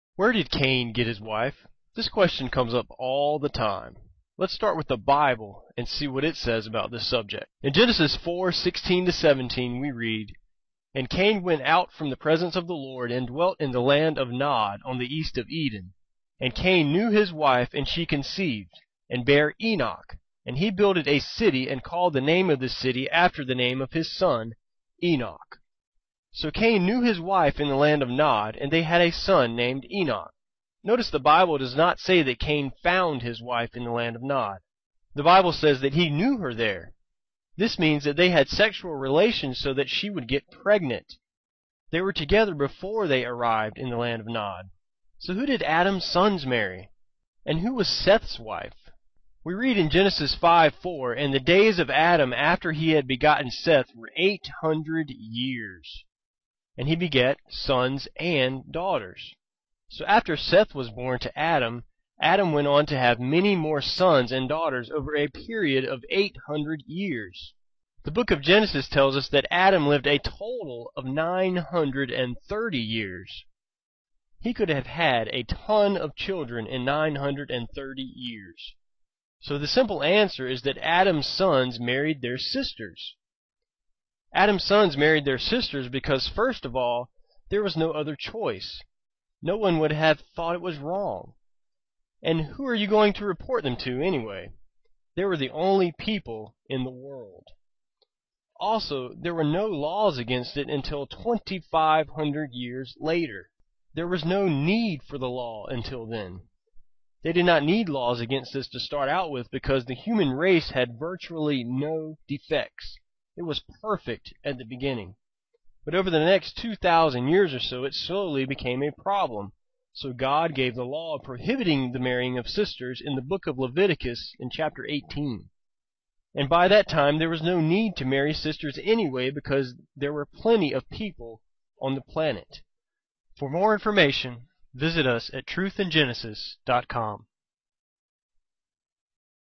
Where did Cain wife came from?
This question always ask never given a perfect answers but let's see.